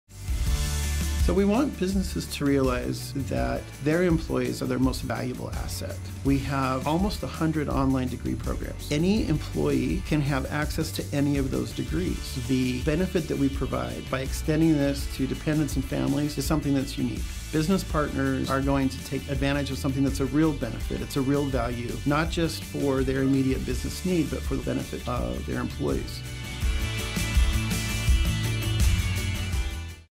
University of Cincinnati Online Business Partnerships | Dean Lemon
At the University of Cincinnati Online, we want to help develop and retain your employees through our Business Partnership offering. A partnership with the University of Cincinnati Online includes a tuition scholarship you can offer employees and their qualified family members in your benefit package.